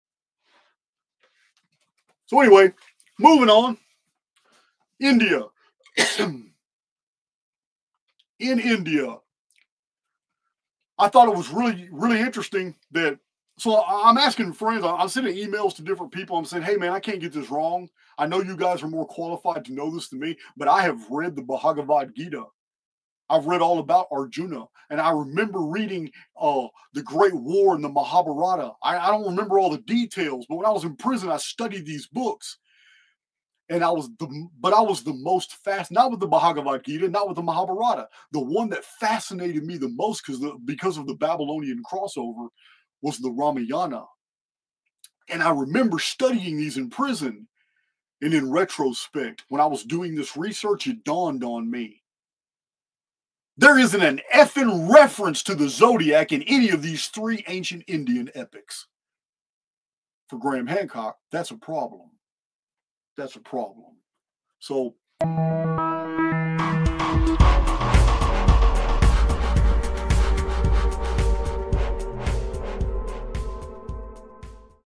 Archaix ∆ Zodiac in India?, Bhagavad Gita, Mahābhārata, Ramayana, No Reference in These Ancient...
Video Source: 52:30-53:35

Archaix ∆ Zodiac in India?, Bhagavad Gita, Mahābhārata, Ramayana, No Reference in These Ancient Epics, Problem for Some

~ ∆RCHAIX SHORTS